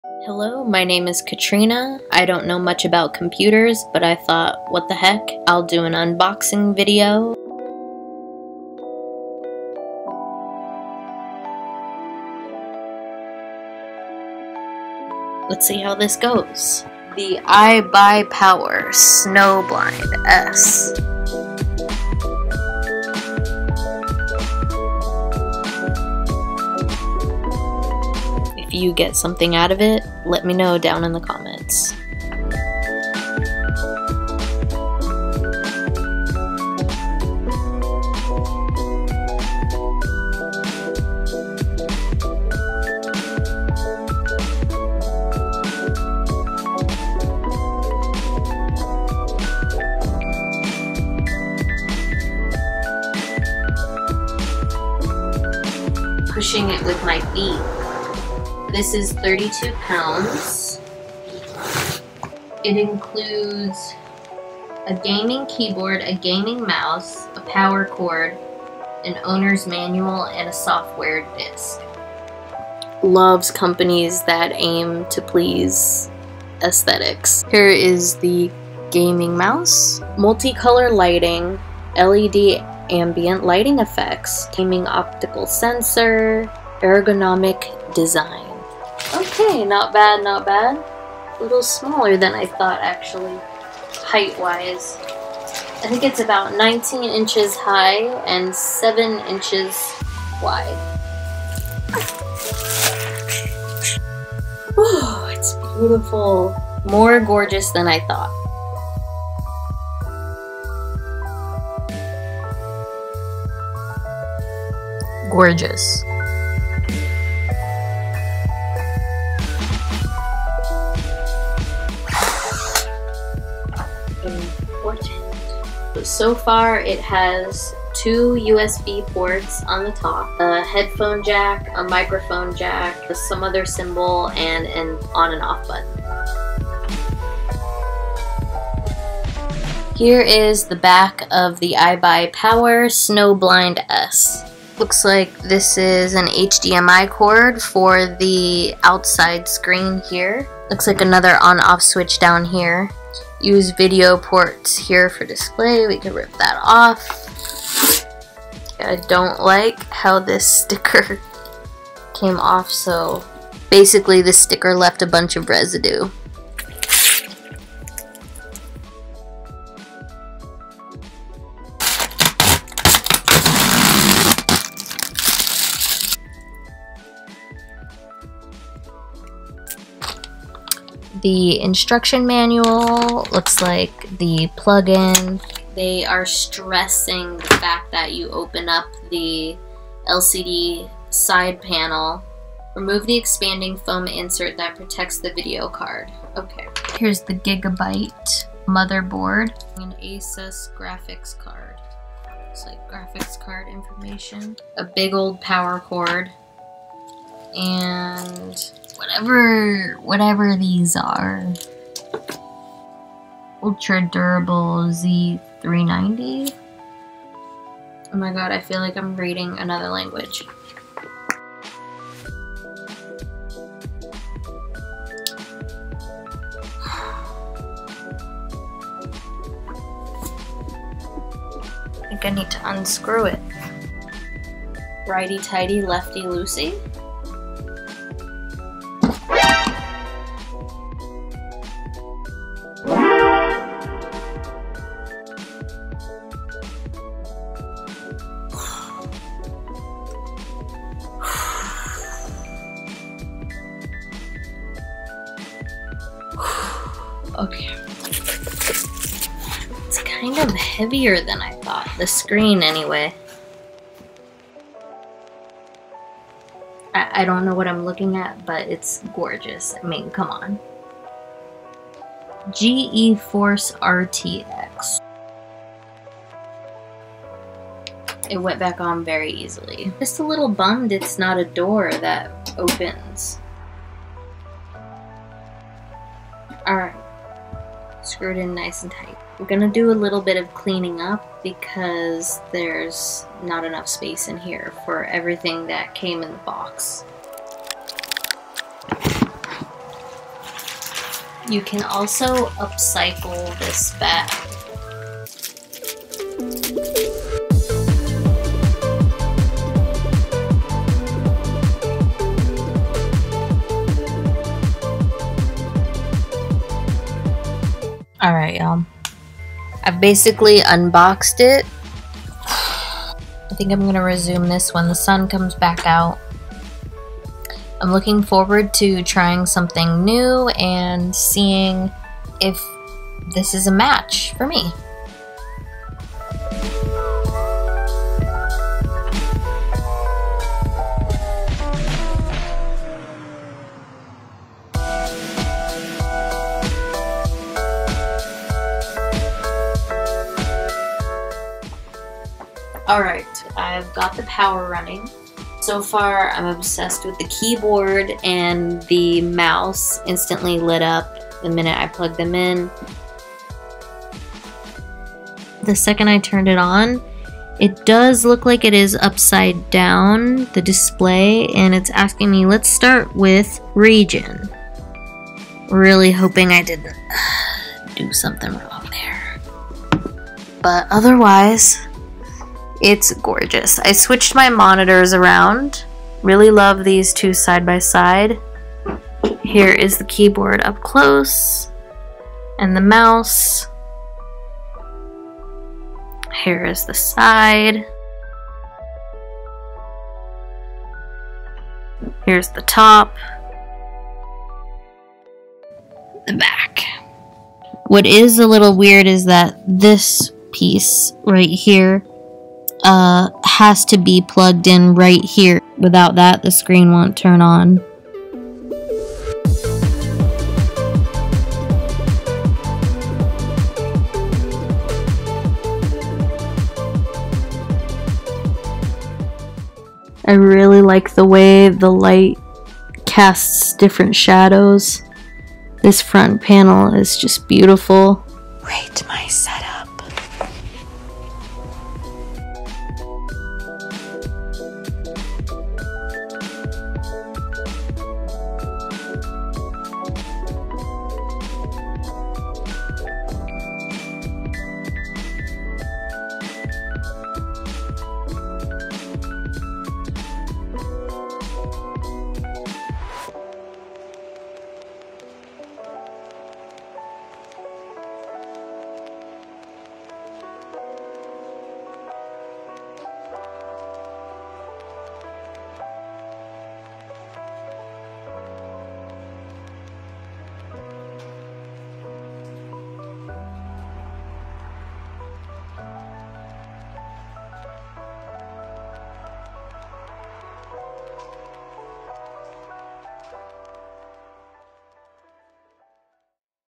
i almost shattered my new IBUYPOWER SNOWBLIND S gaming pc | DWARFtv
KueenKatrina here. This unboxing video was a goal of mine after purchasing the IBUYPOWER Snowblind S NVIDIA GeForce RTX 2080 SUPER during the height of the pandemic 2020.

I can rarely find people with Dwarfism/disabilities posting unboxing videos, I was having fun, and I thought this video might help someone if they're trying to decide on what computer to purchase.

Thank you to IBUYPOWER, BEST BUY, and all of my friends and family who support my Twitch channel and my love for tech items.